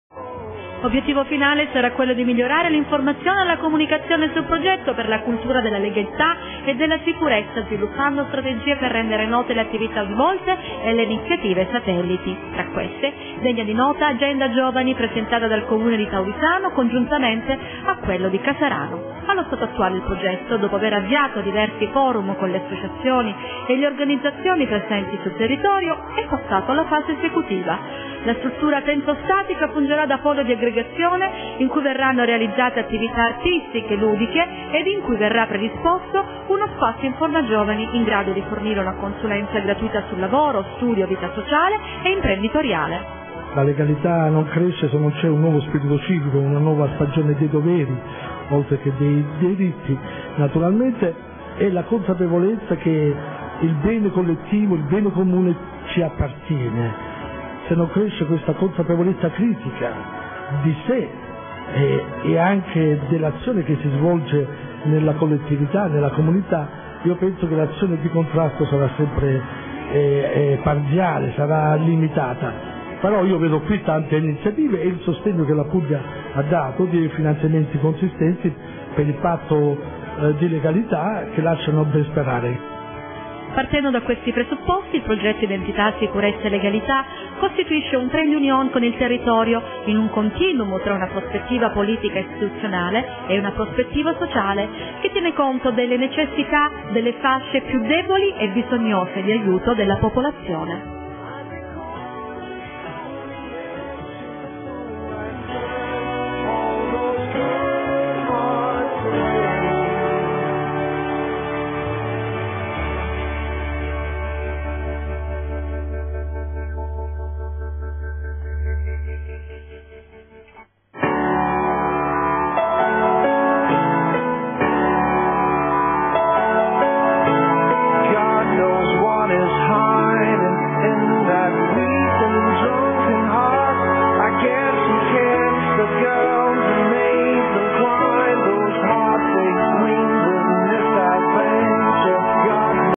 PROGETTO IDENTITA' SICUREZZA & LEGALITA'
- SPECIALE SU CONVEGNO INIZIALE SU LEGALITA' SICUREZZA & POLITICHE GIOVANILI (17.12.07) - QUINTA PARTE (FINALE)- INTERVENTO ASSESSORE REGIONALE FRISULLO